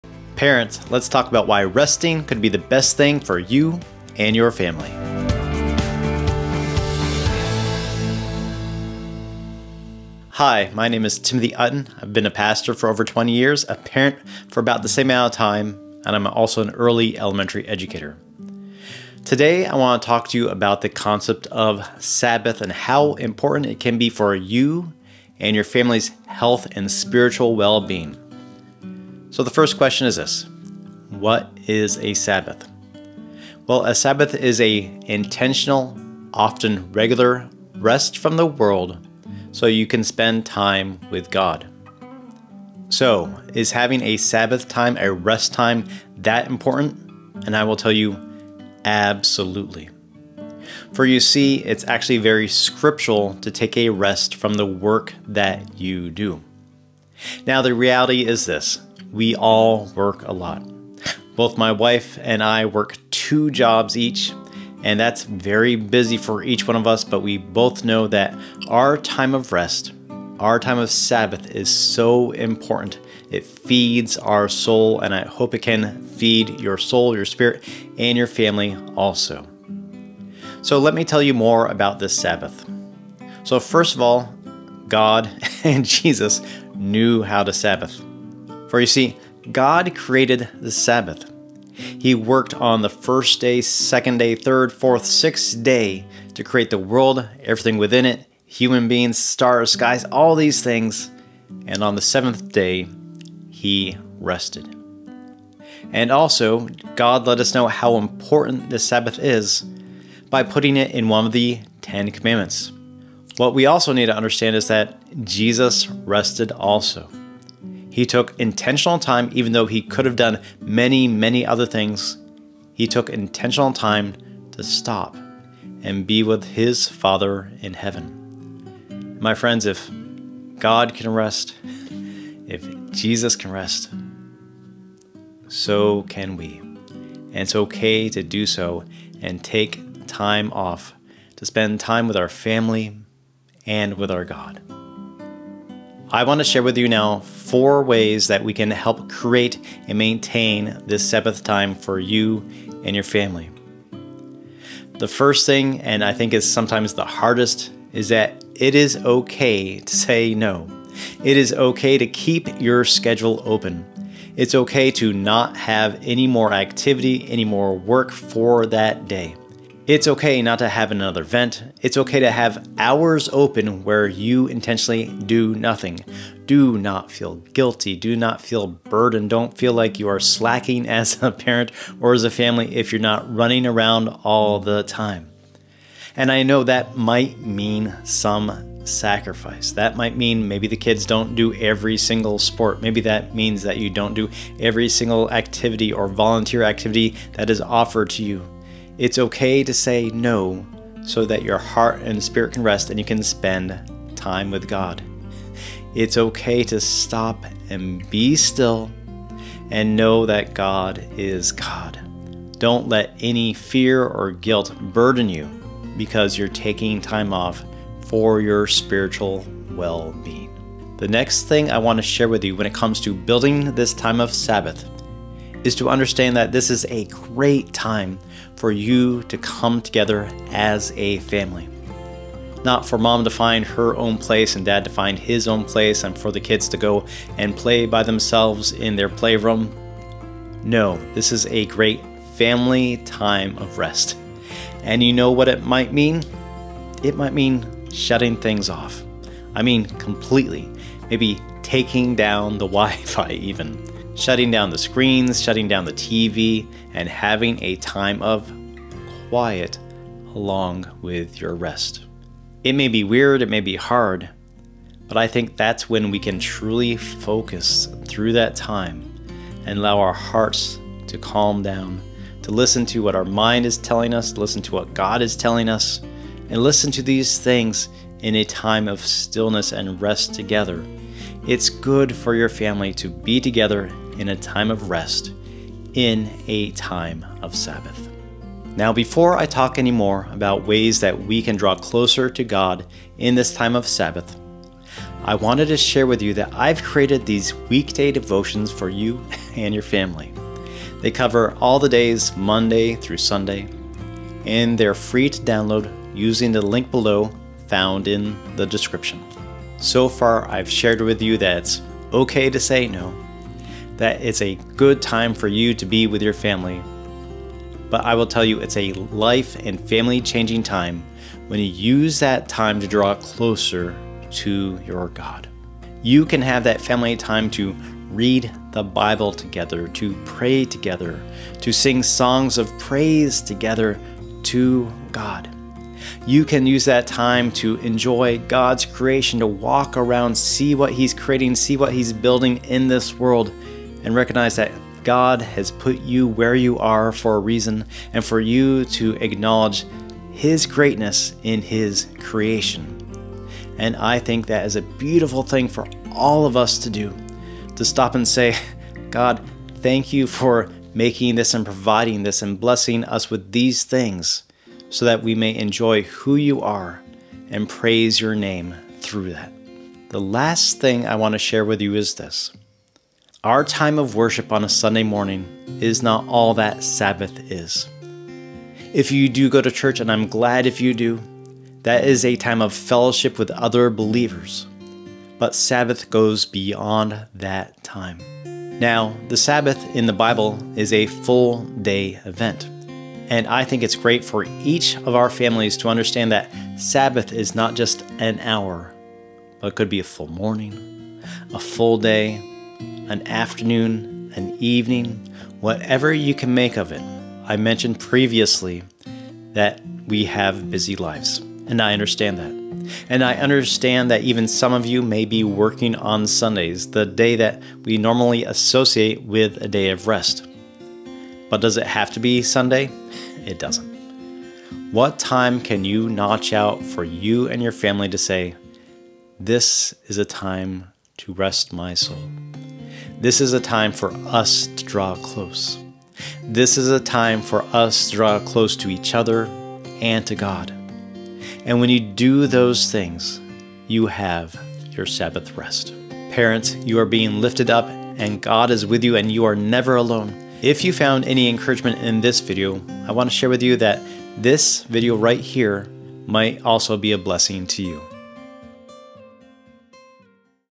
The Life-Changing Gift of the Sabbath For Your Family!
Today Timothy shares about the importance and need of Sabbath rest while also taking some practical steps as a family to make the most of the time.

Support LiftedUp Media:
If you enjoyed the video, please consider liking, commenting, and sharing with friends.

Children's Books Written by Timothy Utton